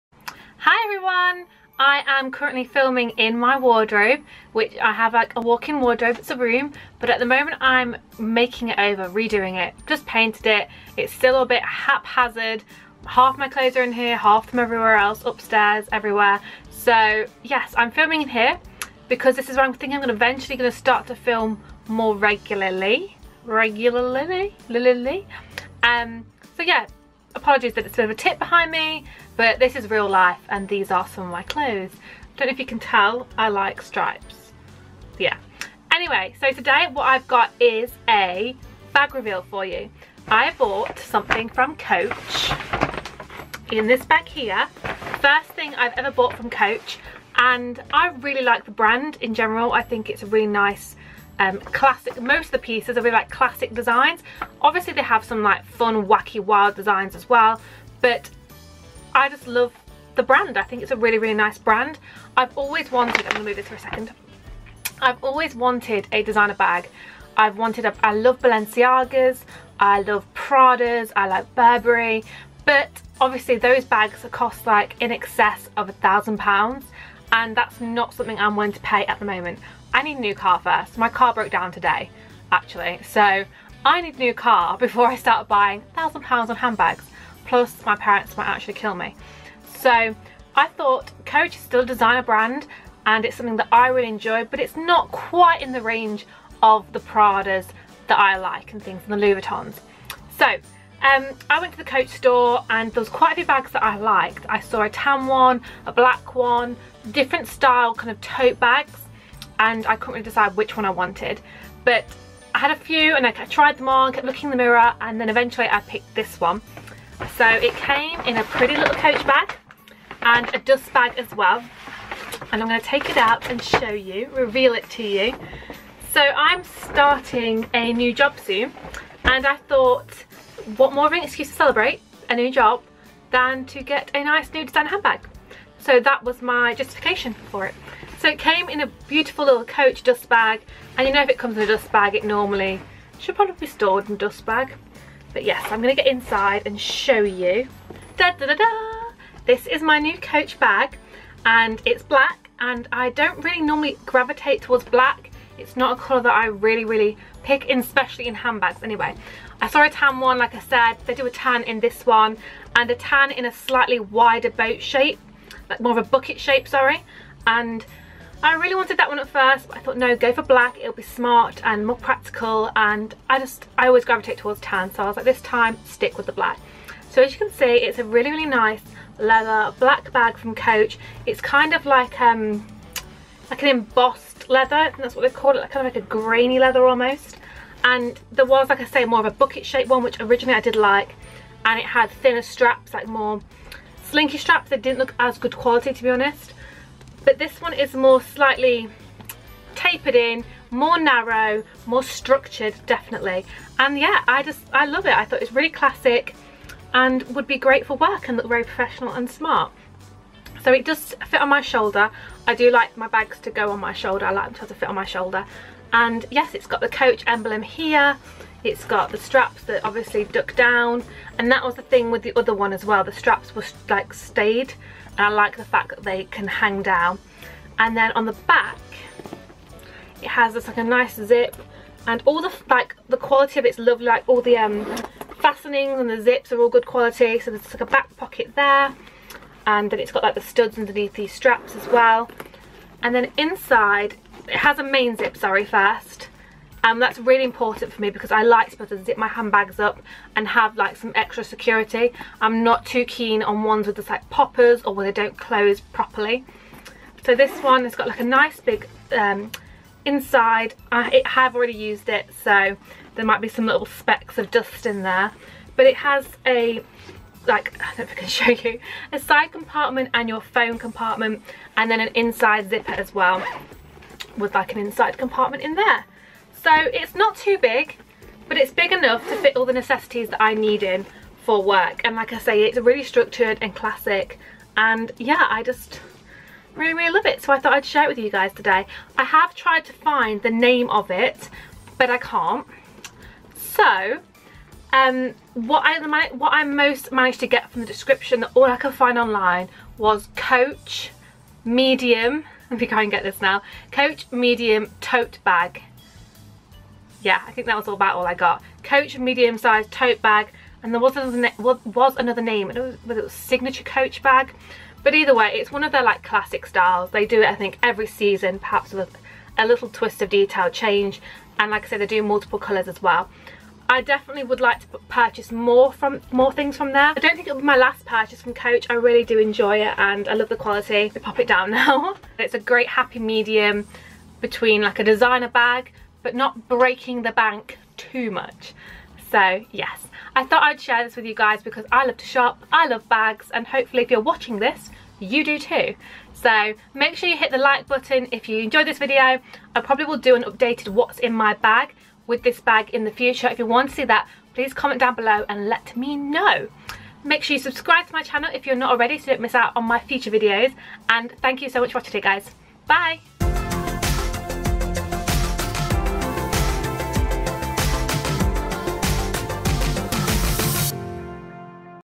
UNBOXING COACH DESIGNER BAG 2017
Designer bag unboxing video I will be showing you my recent Coach Designer bag purchase. I hope you enjoy this unboxing video, and I assume if you are watching you also appreciate the odd designer bag. If you want to see more unboxing type videos please give the video a THUMBS UP! so that I know you LIKE it!

S N A P C H A T: frances_alicia

See you soon xxx